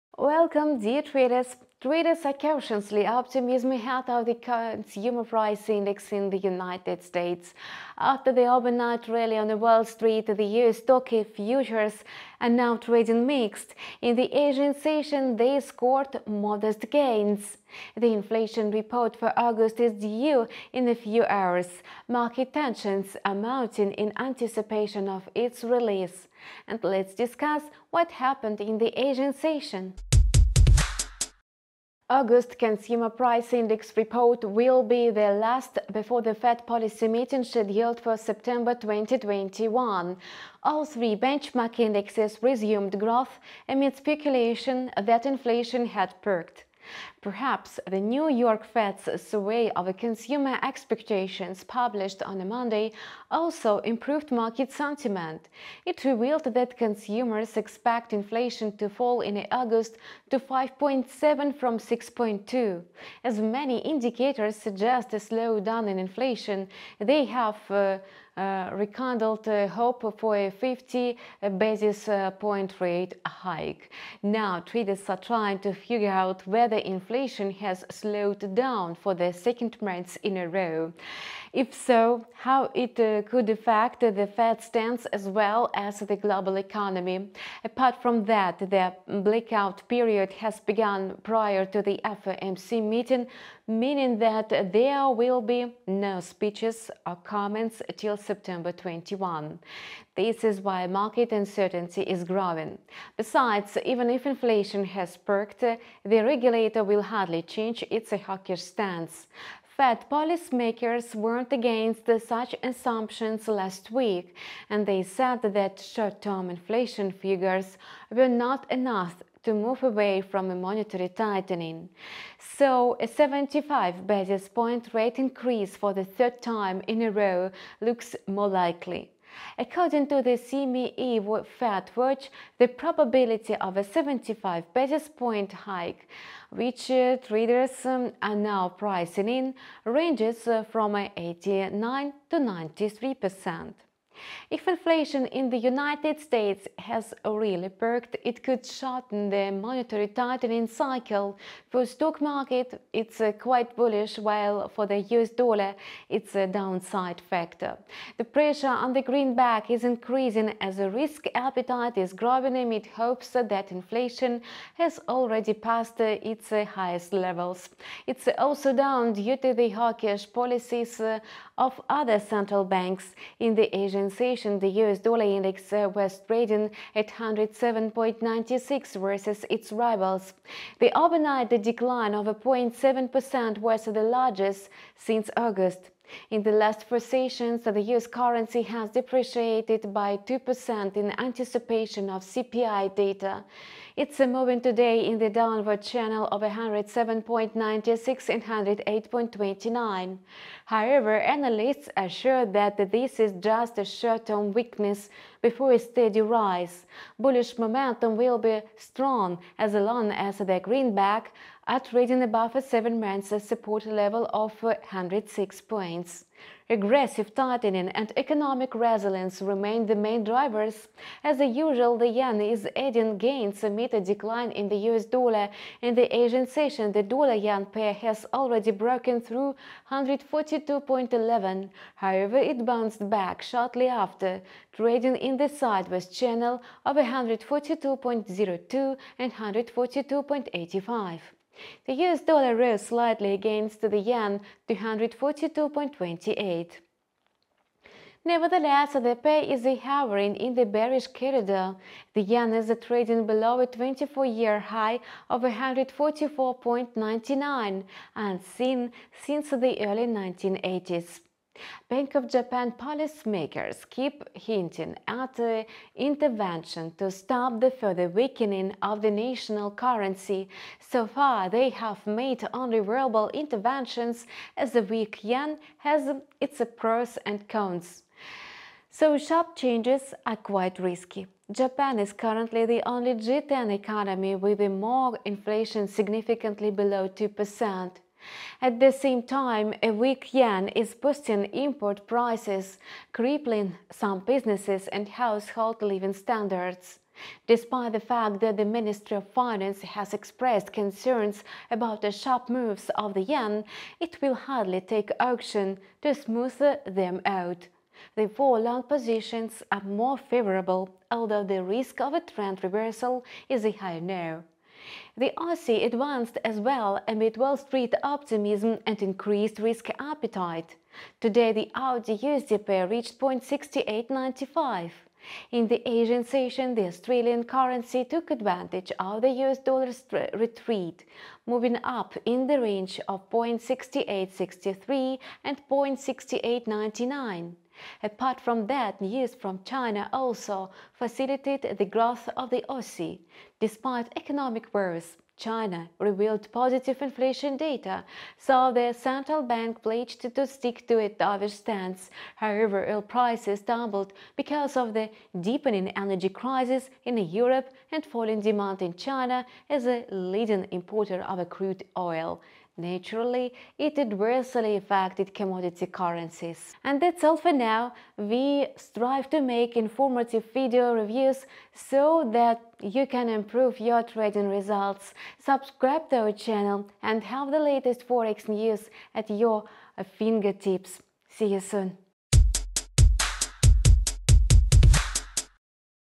13.09.2022: Investors cautious ahead of CPI report; outlook for USDX, USD/JPY, AUD/USD
00:00 INTRO
00:31 CPI DATA EXPECTATIONS
02:24 USDX
03:41 USD/JPY
05:14 AUD/USD